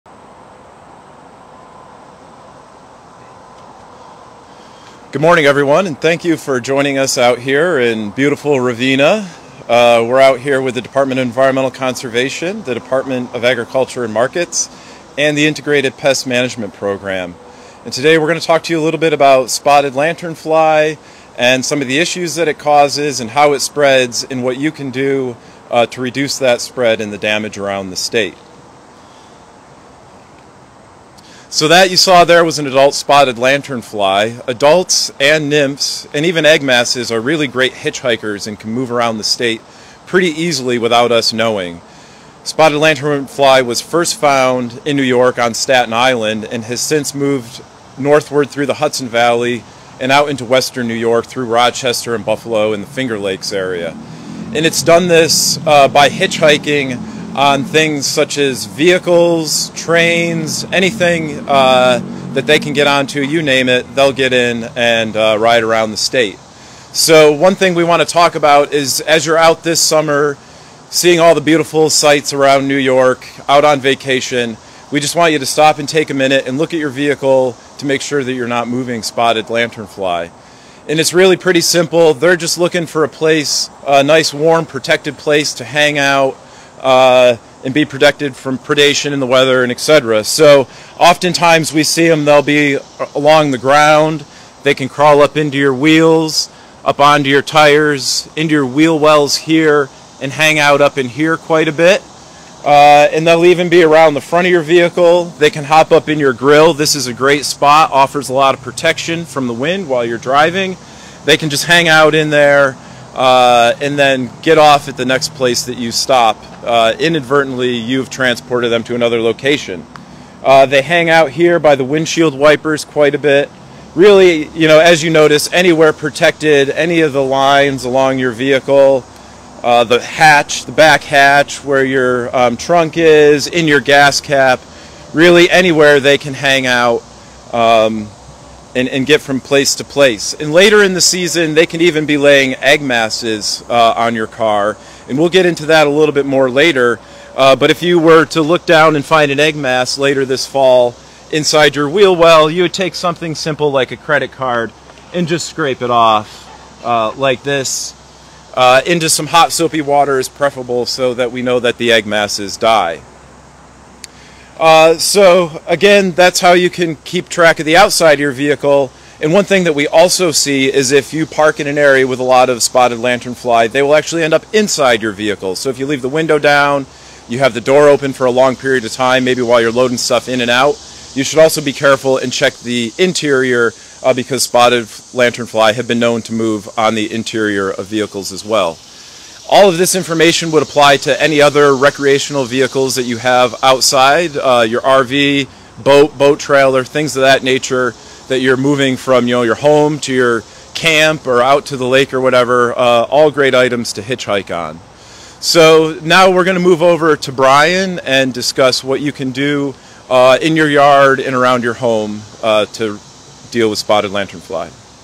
How to Inspect Your Vehicle for Spotted Lanternfly, presented by NYSDAM, NYSDEC, and NYSIPM
A NYSDEC State Forester explains how to inspect your vehicle for spotted lanternfly adults and nymphs when visiting infested areas.